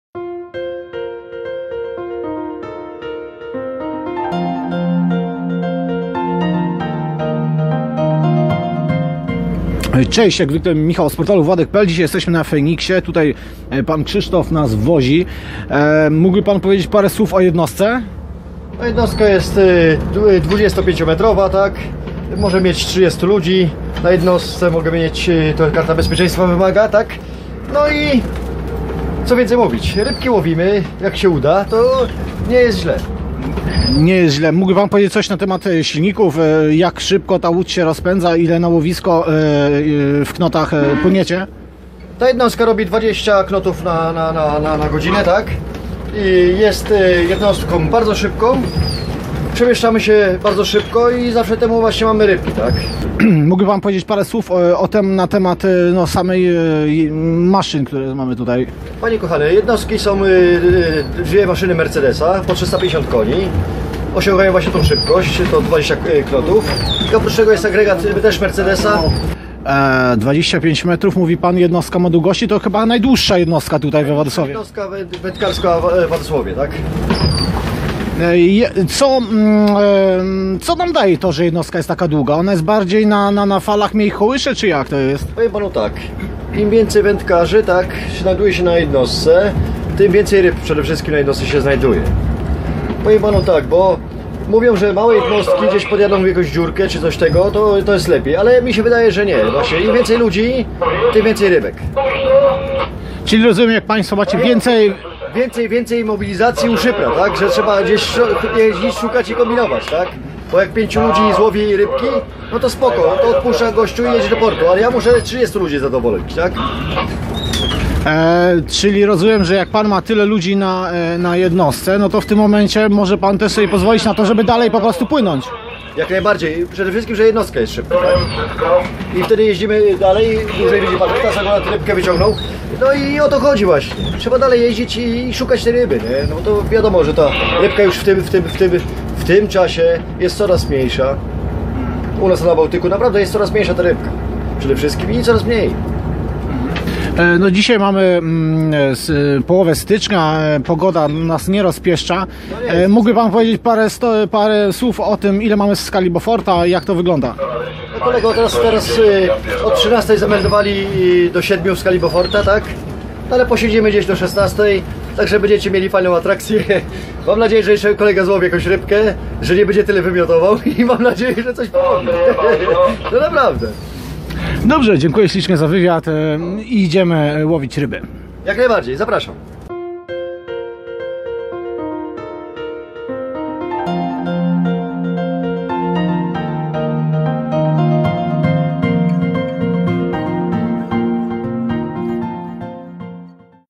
WladekTV - Wywiad z szyprem kutra FENIKS
Tym razem gościmy na jednostce FENIKS. Choć do filmy użyłem słonecznych kadrów uwierzcie mi, że strasznie bujało :) W przygotowaniu jeszcze 3 filmy z tej jednostki. Pozdrawiam serdecznie